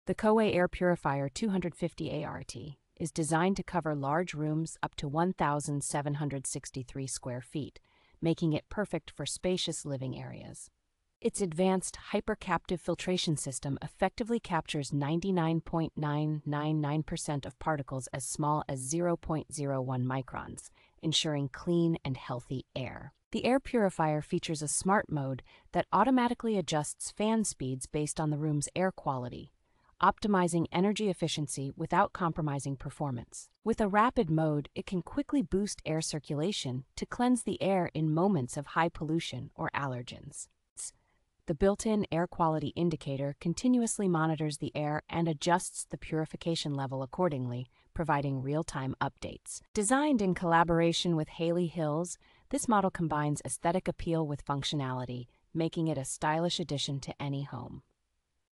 REVIEW (2025): COWAY Air Purifier 250ART(GB). ESSENTIAL details.
The COWAY Air Purifier 250ART(GB) is designed to cover large rooms up to one thousand seven hundred sixty-three square feet, making it perfect for spacious living areas.

Its advanced HyperCaptive Filtration System effectively captures ninety-nine point nine nine nine percent of particles as small as zero point zero one microns, ensuring clean and healthy air.

The air purifier features a Smart Mode that automatically adjusts fan speeds based on the room's air quality, optimizing energy efficiency without compromising performance.

With a Rapid Mode, it can quickly boost air circulation to cleanse the air in moments of high pollution or allergens.

The built-in Air Quality Indicator continuously monitors the air and adjusts the purification level accordingly, providing real-time updates.

Designed in collaboration with Haily Hills, this model combines aesthetic appeal with functionality, making it a stylish addition to any home.